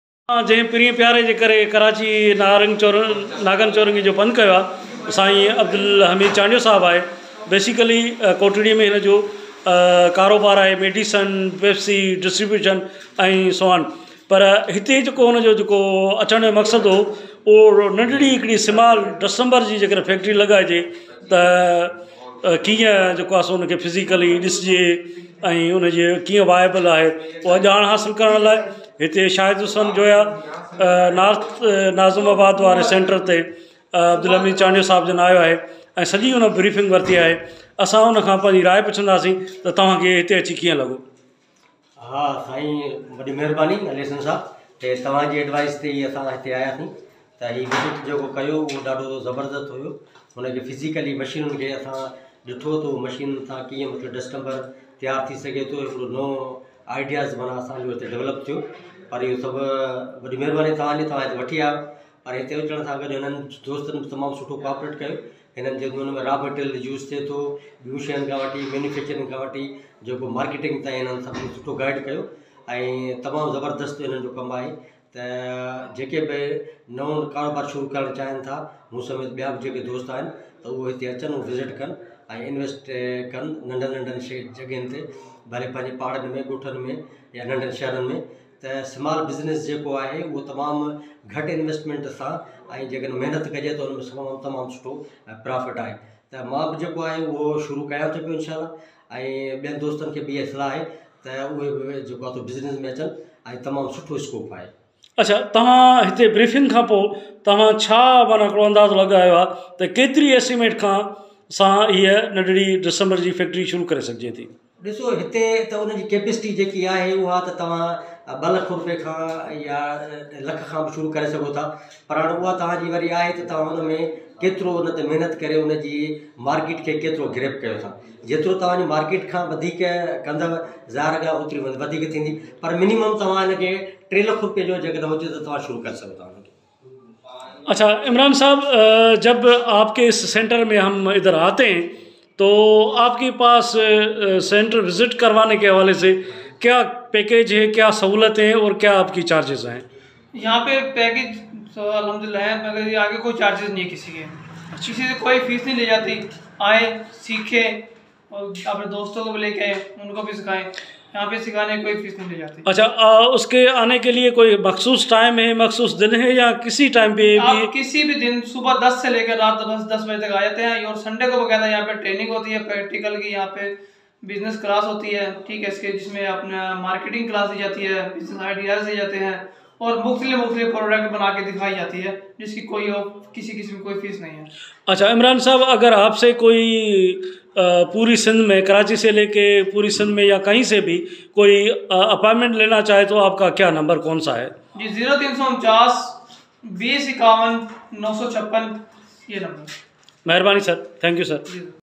famous business tycoon kotri speaks about business centre Karachi
famous business tycoon of kotri Mr Abdul Hamid Chandio speaks about Shahid hussain joya business centre Karachi